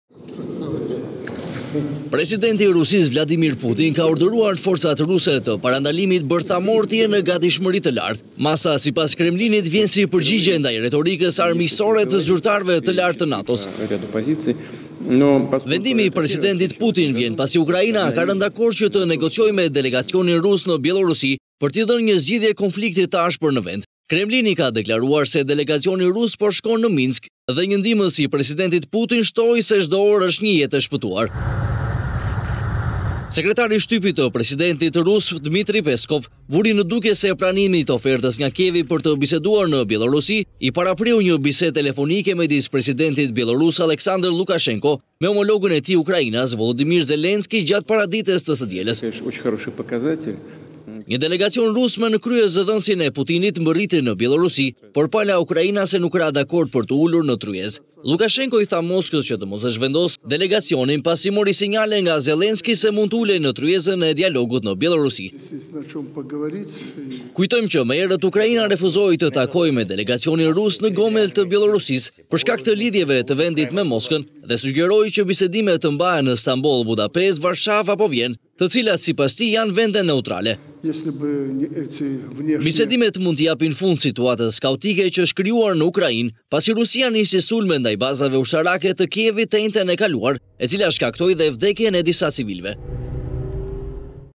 Rusia në alarm nga sulmet e NATO-s, urdhëron forcat ruse bërthamore të jenë në gatishmëri
VIDEO Credits: AFP

Ukraina dhe Rusia duket se kanë rënë dakord për të biseduar në Bjellorusi, ftesa e Moskës u refuzua përkohësisht nga presidenti Zelenski, por me ndërmjetësimin e Lukashenkos Kievi tha se ka rënë dakord për t’u takuar me delegacionin rus, çka mund t’i jepte fund tensioneve në lindje të Evropës.

Presidenti i Rusisë, Vladimin Putin ka urdhëruar forcat ruse të parandalimit bërthamor të jenë në gatishmëri të lartë.

Masa sipas Kremlinit vjen si përgjigje ndaj retorikës armiqësore të zyrtarëve të lartë NATO-s.

Vendimi i presidentit Putin vjen pasi Ukraina ka rënë dakord që të negociojë me delegacionin rus në Bjellorusi për t’i dhënë një zgjidhje konfliktit të ashpër në vend.

Kremlini ka deklaruar se delegacioni rus po shkon në Minsk, një ndihmës i presidentit Putin tha se çdo orë është një jetë e shpëtuar.

Sekretari i shtypit i Presidentit të Rusisë, Dmitry Peskov vuri në dukje se pranimit të ofertës nga Kievi për të biseduar në Bjellorusi i parapriu një bisedë telefonike midis presidentit bjellorus Aleksandër Lukashenko me homologun e tij ukrainas Vollodimir Zellenski gjatë paradites së të dielës.

Një delegacion rus me në krye zëdhënësin e Putinit mbërriti në Bjellorusi, por pala ukrainase nuk ra dakord për t’u ulur në tryezë.

Lukashenko i tha Moskës që të mos e zhvendoste delegacionin pasi mori sinjale nga Zelenski se mund të ulej në tryezën e dialogut në Bjellorusi.

Kujtojmë që më herët Ukraina refuzoi të takohej me delegacionin rus në Gomel të Bjellorusisë për shkak të lidhjeve të vendit me Moskën dhe sugjeroi që bisedimet të mbahen në Stamboll, Budapest, Varshavë, Vjenë apo në vende të tjera neutrale.

Bisedimet mund t’i japin fund situatës kaotike që është krijuar në Ukrainë, pasi Rusia nisi sulmet ndaj bazave ushtarake të Kievit të enjten e kaluar e cila shkaktoi edhe vdekjen e disa civilëve.